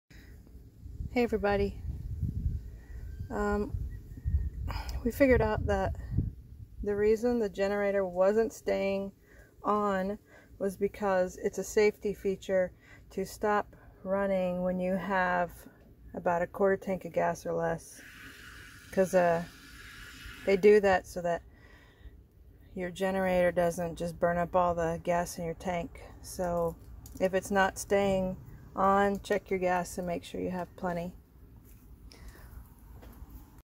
Retro 90's RV🚍 Bounder generator issue? You may be low on gas.
Hi 1st time RVers if your generator wont stay on make sure you have enough gas. Onan is designed to shut off if your gas is low👍🙋‍♀️